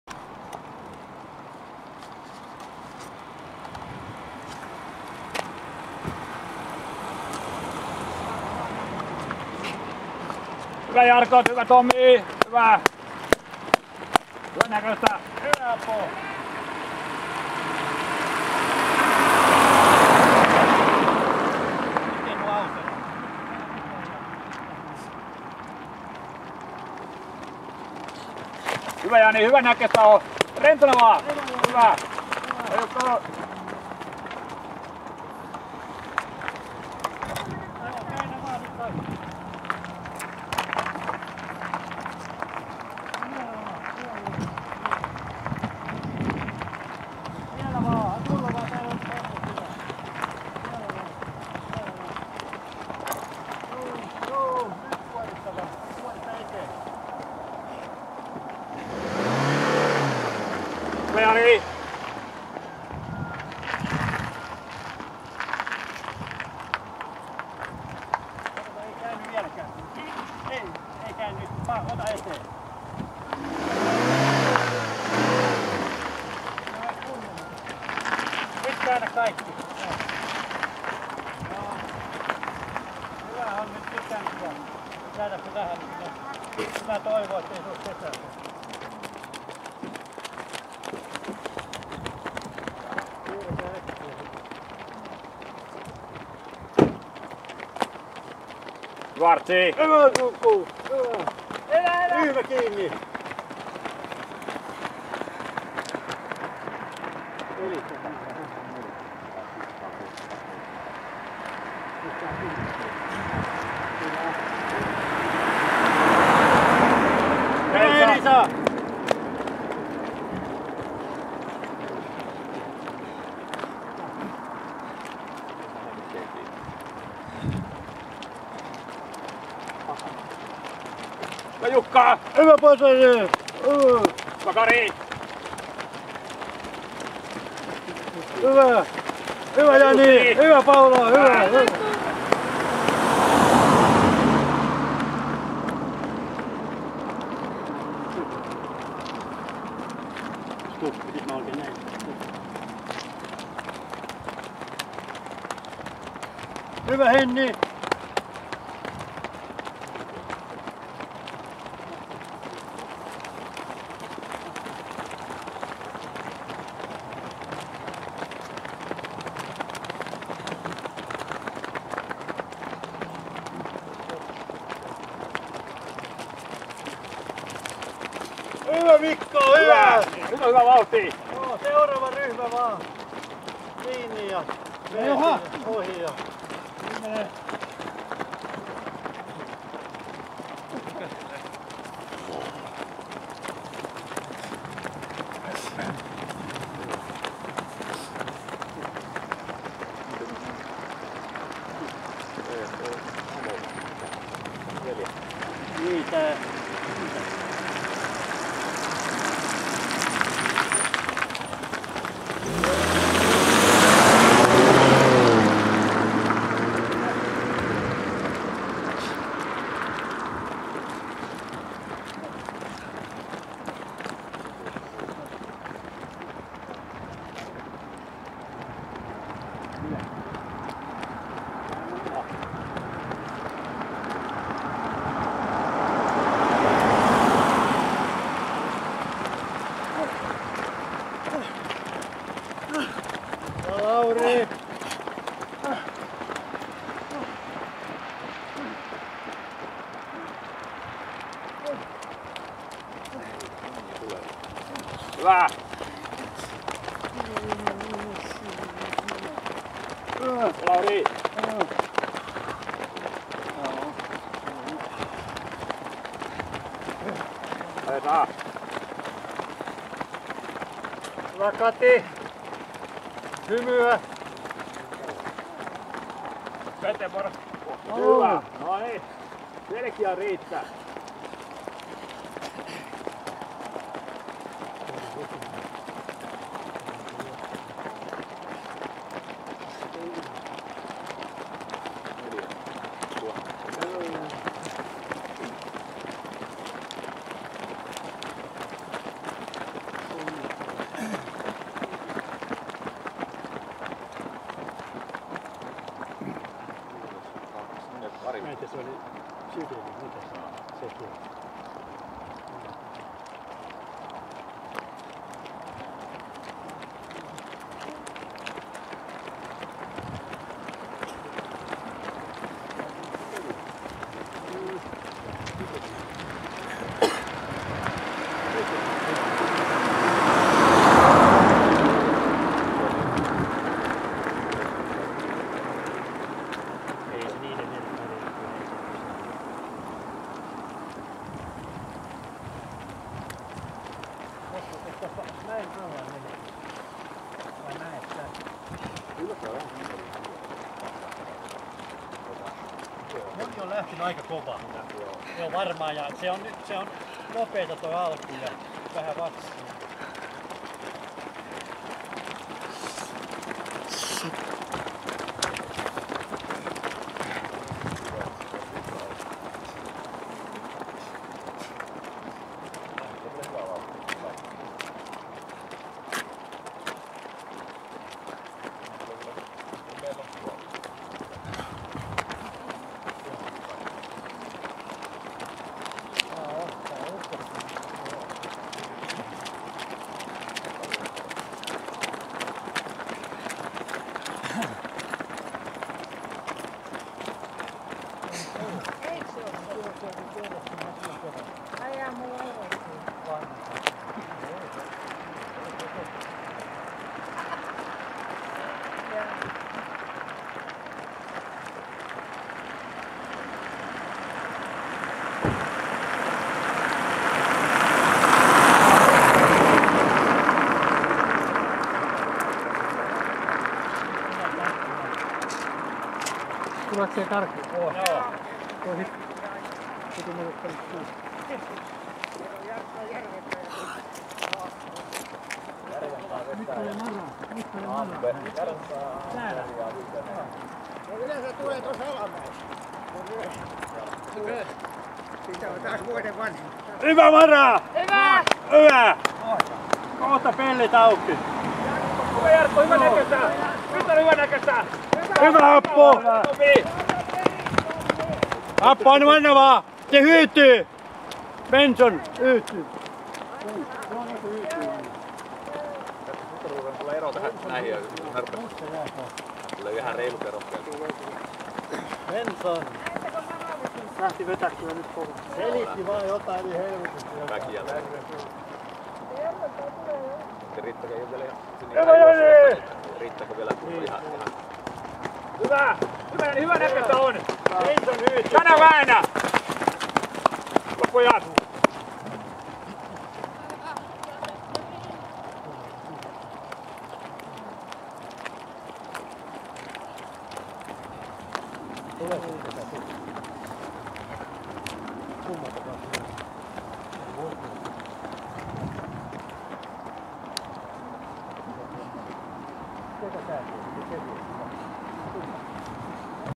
Aktia cup 15.11.2014 4 km
Aktia maantiejuoksucupista kuvaa hieman ennen 4 km: täyttymistä